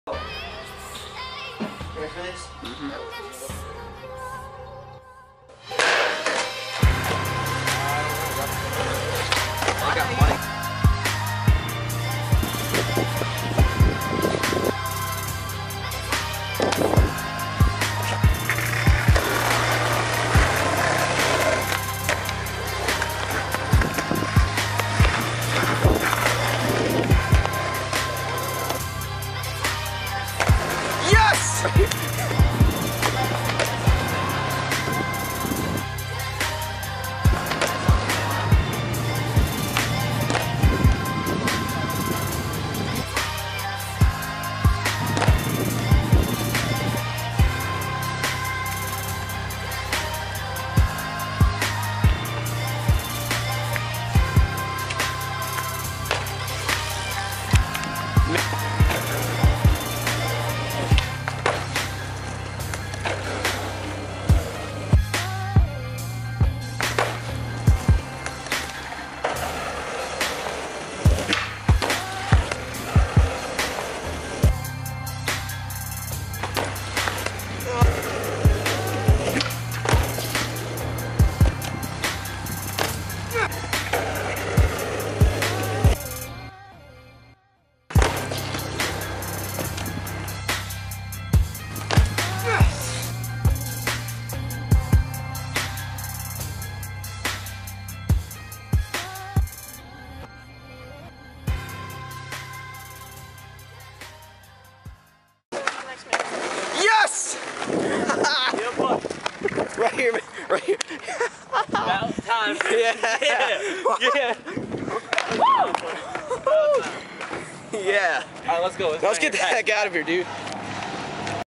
Last Sunday Skateboard Edit
Had a crazy fun time last Sunday got some footy for my video part!
I'm still recovering a little bit though cause when I was trying to tre of that dock I took a bad shinner.